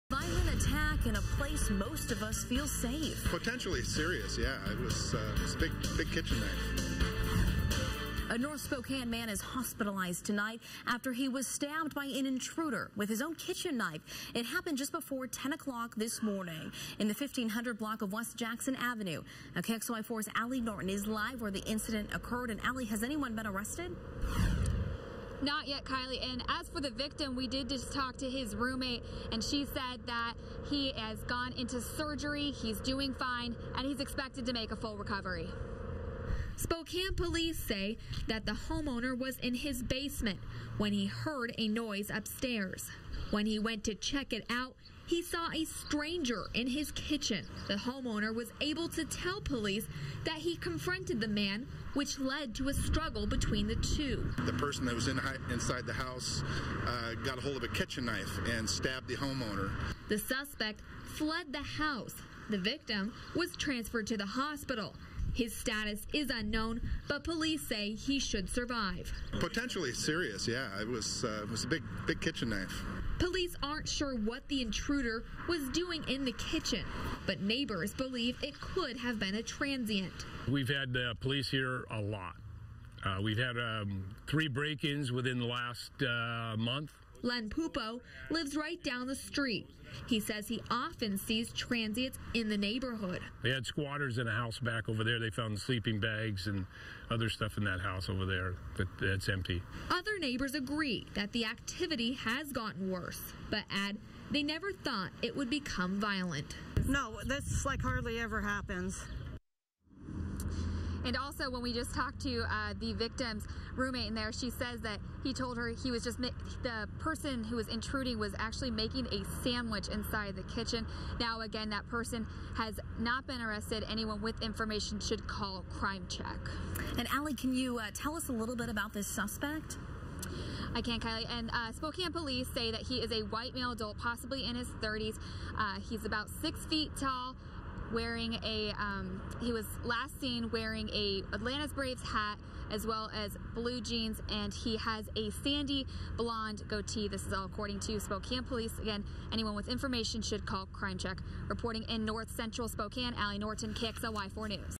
Homeowner stabbed during home invasion robbery
Spokane Police are investigating a stabbing that happened at a residence near the intersection of Maple Street and Jackson Avenue Monday morning. KXLY4's Allie Norton reports.